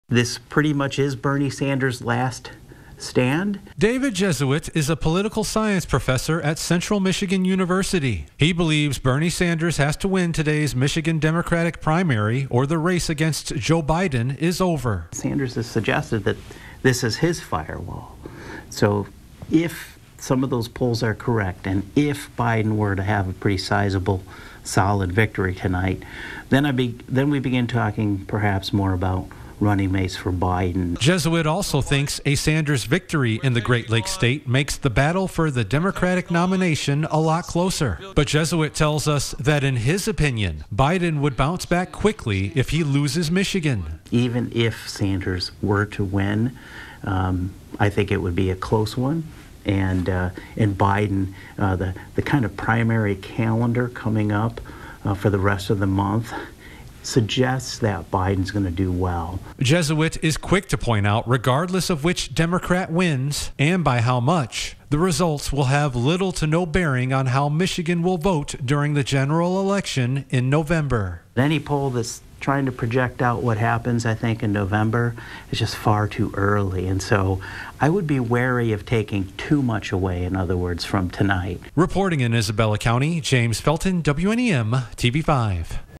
Political expert predicts Michigan is Sanders' last chance
As Michigan finishes at the polls Tuesday, an expert says it will likely be Bernie Sanders’ last hope.